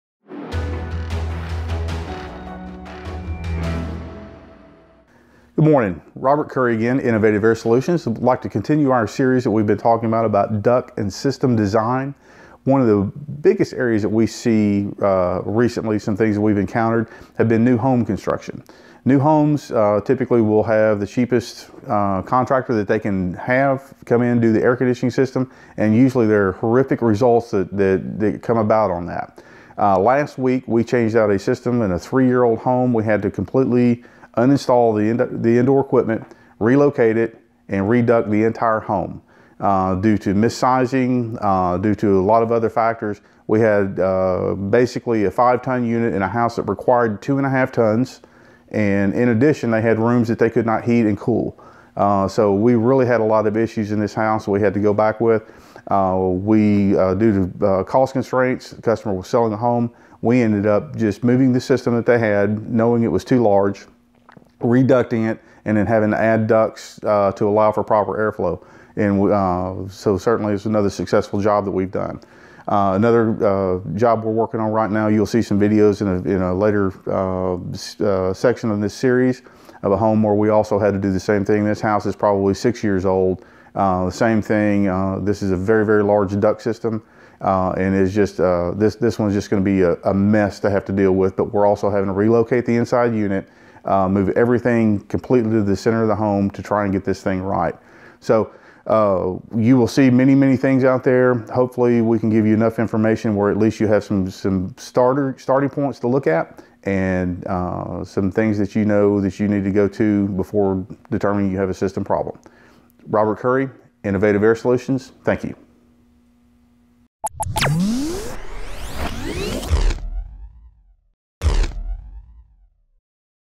Understanding HVAC for New Home Construction
Many times in new home construction the contractor will go with the lowest bid to install the Air System.  Too many times they end up with bad results. Correct installation and a properly sized unit are key in installing a system that will run effectively and save money.

In this CTE video, Robert Currie with Innovative Air Solutions explains some of the key factors in getting this right for the customer.